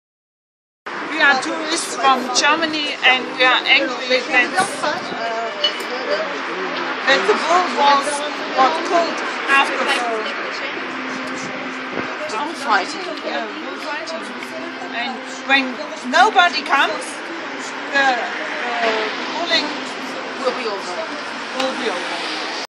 AAT entrevista turistas Alemães/AAT interviews German Turists PRAÇA DE TOUROS ALBUFEIRA
AAT CAMPANHA TURISMO PELA ABOLIÇÃO I German Turists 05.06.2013/Testimony